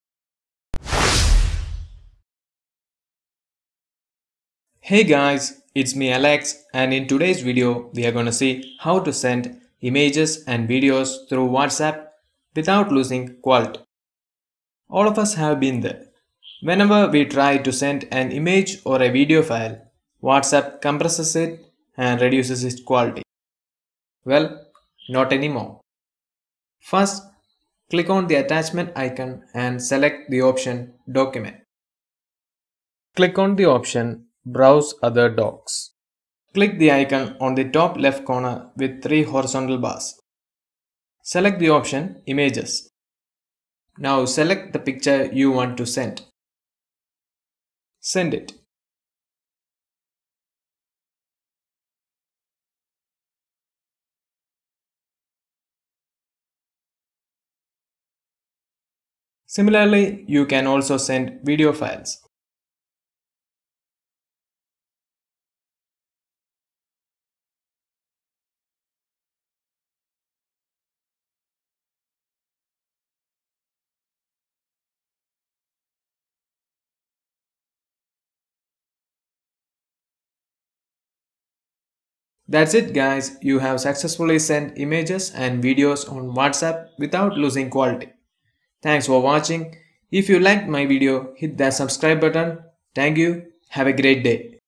How to Send Pictures and Videos on WhatsApp Without Losing Quality
Hey Guys, Today I show you how to send pictures and videos through WhatsApp without losing quality.
(This video is completely in English)
Every time when we send an Image or Video file through WhatsApp, they compress them and thus lose their quality.
Want to send Images and Video with their actual size and quality?
Watch this video and learn how to avoid it. Don't forget to teach your friends too...